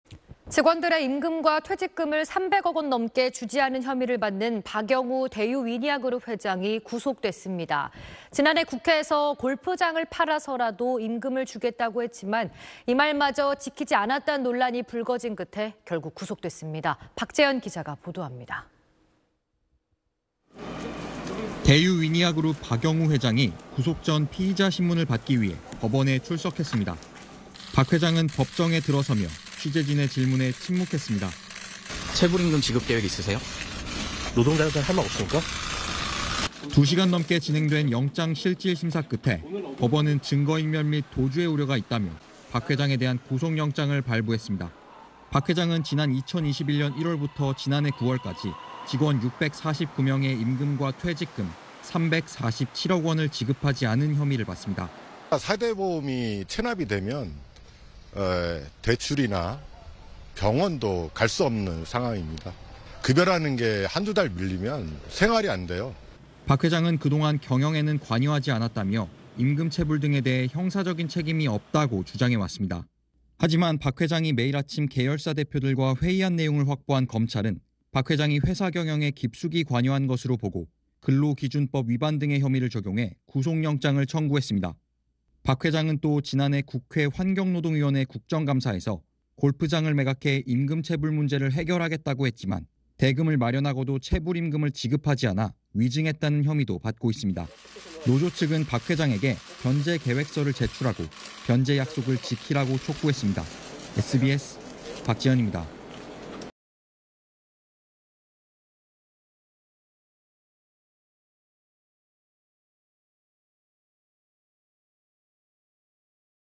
'300억대 임금 체불' 박영우 대유위니아 회장 구속 / SBS
〈앵커〉

직원들의 임금과 퇴직금을 3백억 원 넘게 주지 않은 혐의를 받는 박영우 대유위니아 그룹 회장이 구속됐습니다. 지난해 국회에서 골프장을 팔아서라도 임금을 주겠다고 했지만, 이 말마저 지키지 않았다는 논란이 불거진 끝에 결국, 구속됐습니다.

박재연 기자가 보도합니다.

〈기자〉

대유위니아 그룹 박영우 회장이 구속 전 피의자 심문을 받기 위해 법원에 출석했습니다.

박 회장은 법정에 들어서며 취재진의 질문에 침묵했습니다.

[박영우/대유위니아그룹 회장 : (체불 임금 지급 계획 있으세요?) ……. (노동자들한테 할 말 없습니까?) …….]

2시간 넘게 진행된 영장실질심사 끝에 법원은 "증거 인멸 및 도주의 우려가 있다"며 박 회장에 대한 구속 영장을 발부했습니다.

박 회장은 지난 2021년 1월부터 지난해 9월까지 직원 649명의 임금과 퇴직금 347억 원을 지급하지 않은 혐의를 받습니다.

[남승대/위니아딤채 노조위원장 : 4대 보험이 체납되면 대출이나 병원도 갈 수 없는 상황입니다. 급여라는 게 한두 달 밀리면 생활이 안 돼요.]

박 회장은 그동안 경영에는 관여하지 않았다며 임금 체불 등에 대해 형사적인 책임이 없다고 주장해 왔습니다.

하지만 박 회장이 매일 아침 계열사 대표들과 회의한 내용을 확보한 검찰은 박 회장이 회사 경영에 깊숙이 관여한 것으로 보고 근로기준법 위반 등의 혐의를 적용해 구속영장을 청구했습니다.

박 회장은 또 지난해 국회 환경노동위원회 국정감사에서 골프장을 매각해 임금 체불 문제를 해결하겠다고 했지만, 대금을 마련하고도 체불 임금을 지급하지 않아 위증했다는 혐의도 받고 있습니다.

노조 측은 박 회장에게 변제 계획서를 제출하고 변제 약속을 지키라고 촉구했습니다.

(영상취재 : 양두원, 영상편집 : 최혜란)